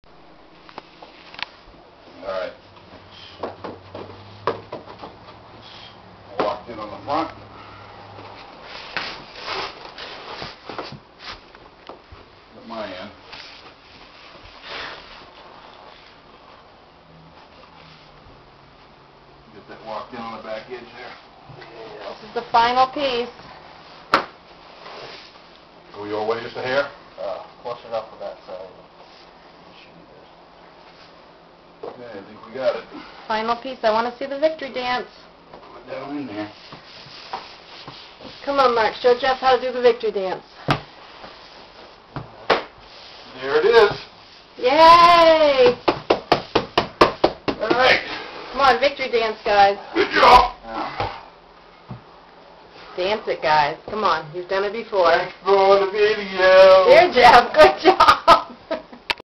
the last piece of flooring!!!
Big Mac andJeff laid down the past piece of the kitchen/dining room flooring at 3:45 p.m. :D  :D  :D